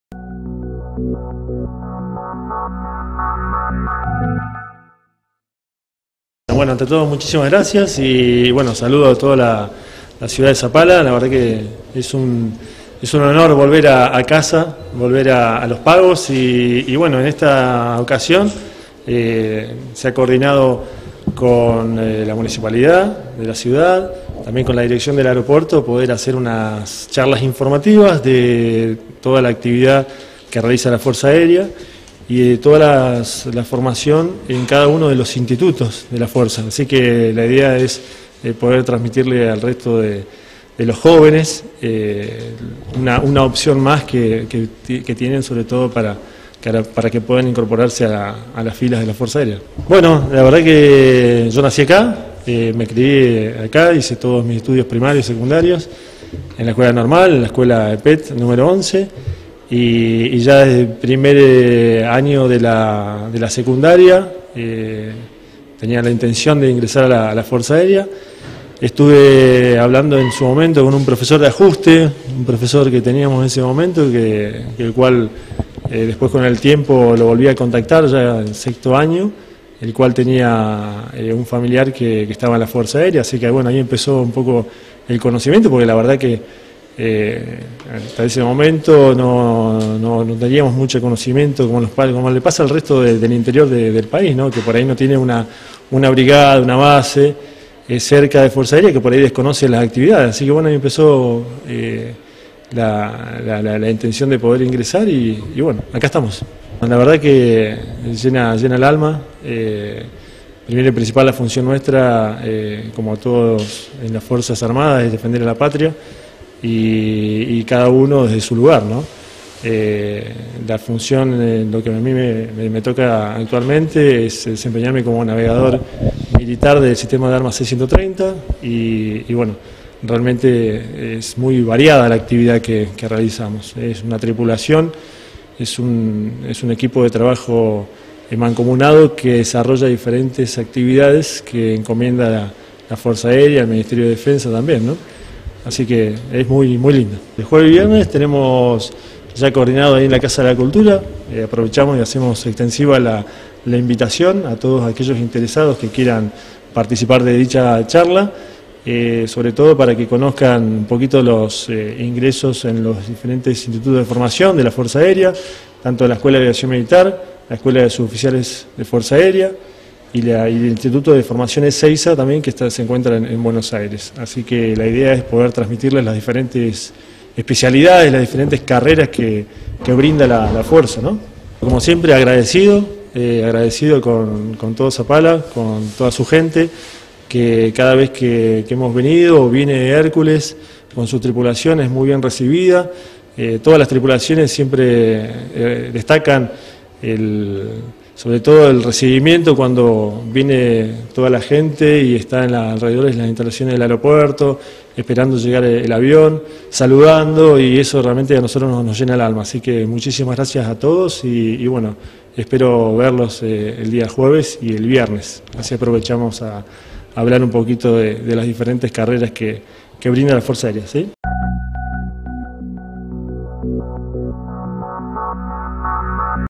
My Pablo Nuñez Oficial Fuerza Aerea
Brindará charlas orientativas de las opciones de formación en la Fuerza Aerea Argentina.
Jueves y Viernes, 10 a 12 hs. Casa de la Cultura Zapala